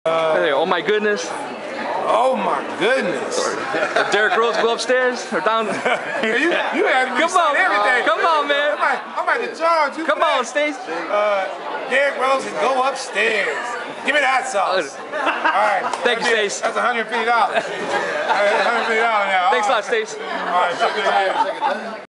Stacey King gives me the Hot Sauce!
Chicago Bull / Announcer: Stacey King gives me the Hot Sauce at a NW Indiana local jewelry store. Next to Stacey is Chicago Bull Bill Wennington.